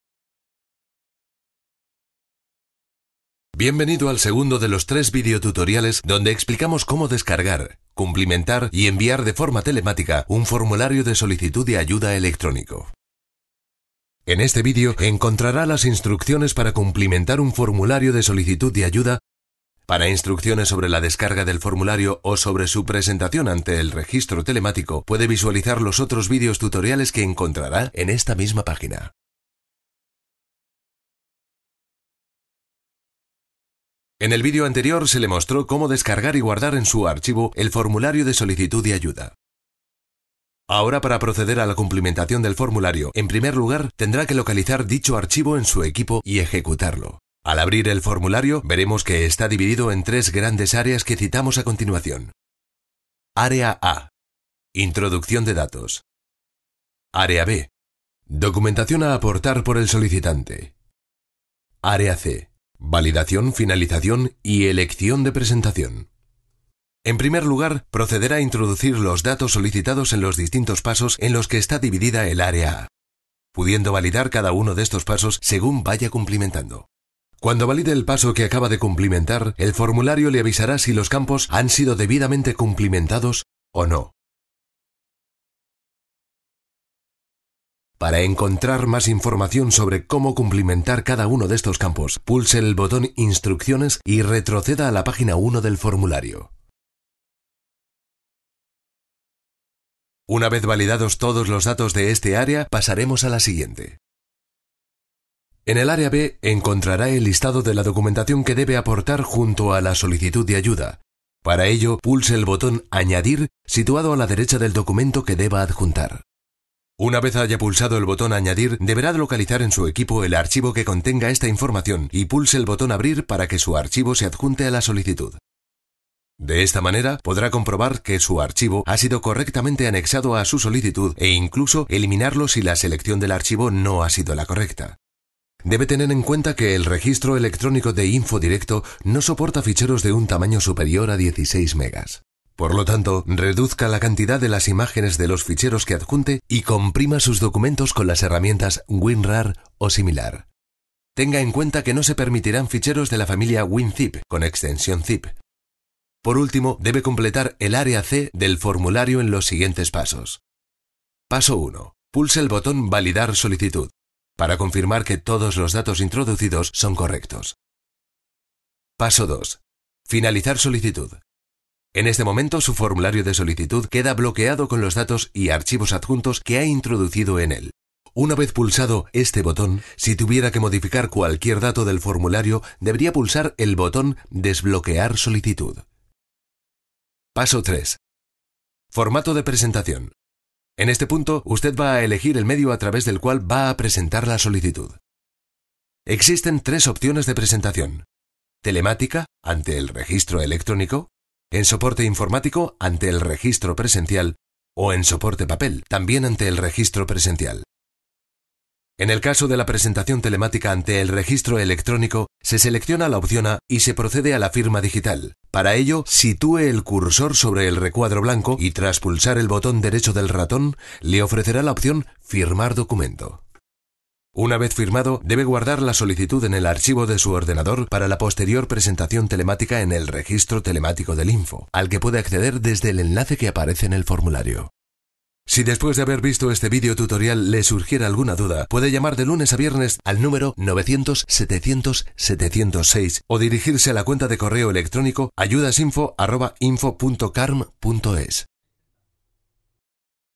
INFOdirecto: Cumplimentación telemática
Vídeo explicativo sobre la tramitación electrónica de ayudas del INFO.
En este segundo vídeo se muestra la forma en la que se puede cumplimentar el formulario de solicitud de ayuda del Instituto de Fomento de la Regióin de Murcia